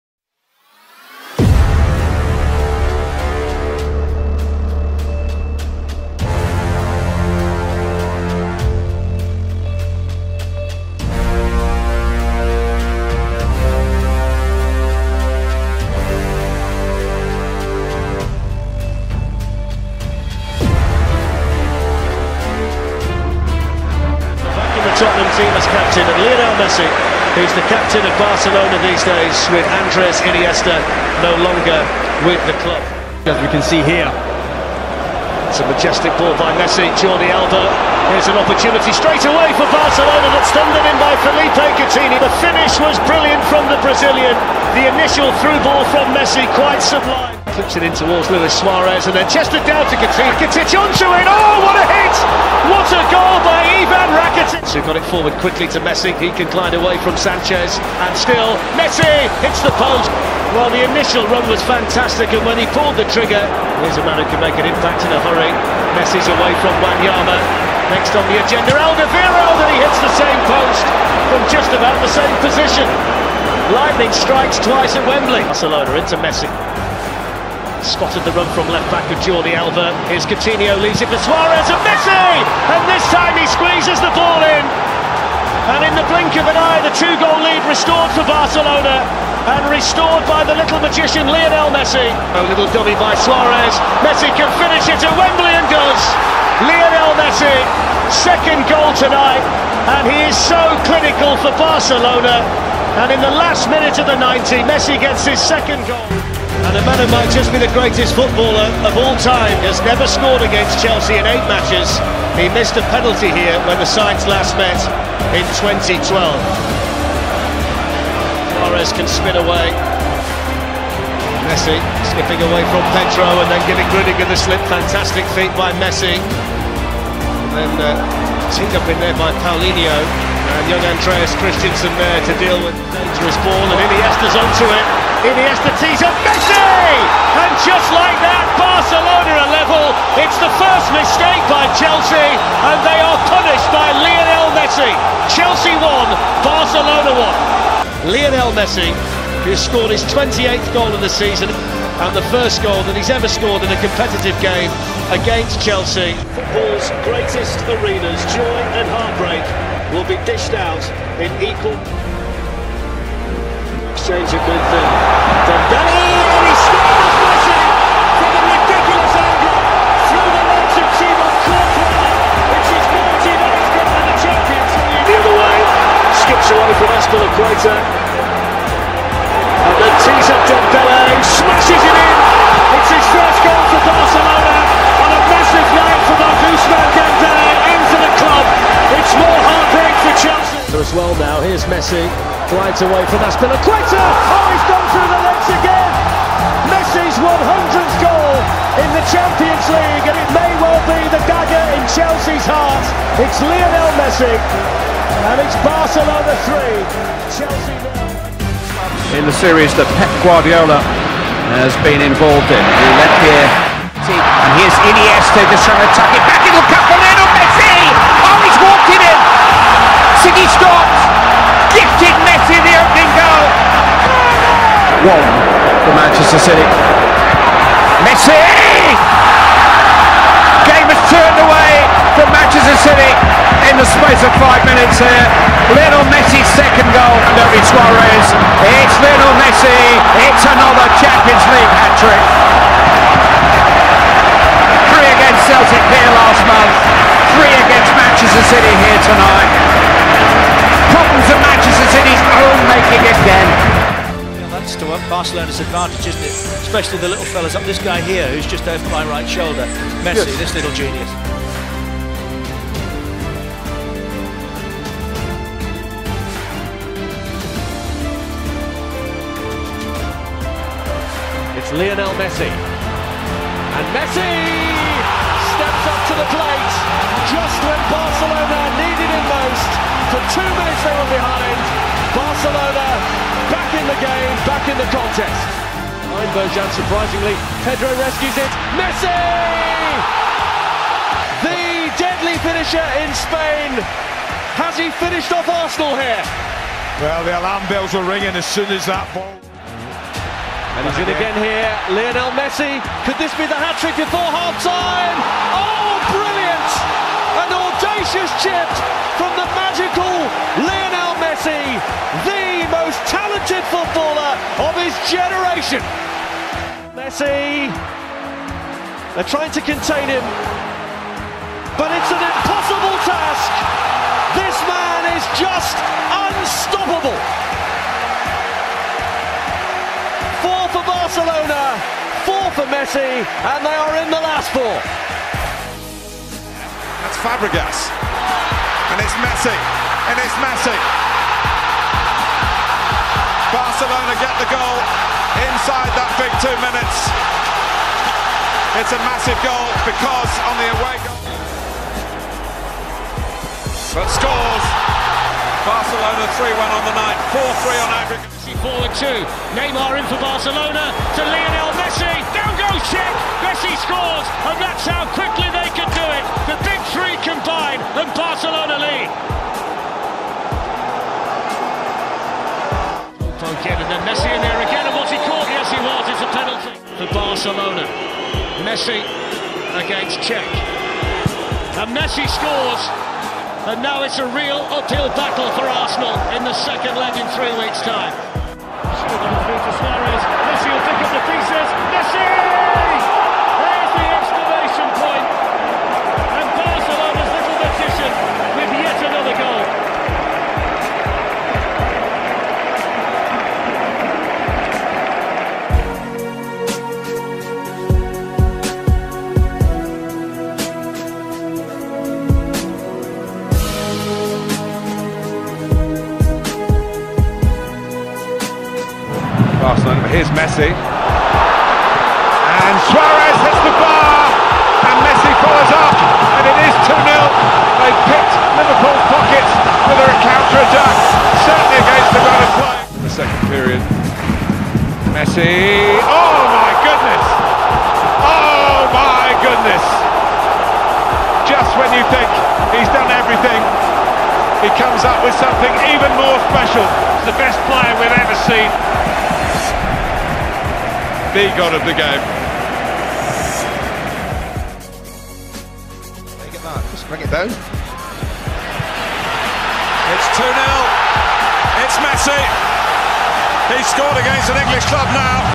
Leo Messi playing against some English Clubs... Watch the performance...
In this video you can watch performance of Lionel Messi against some famous and great English clubs like Arsenal, Chelsea, Tottenham and etc. while playing in Barcelona...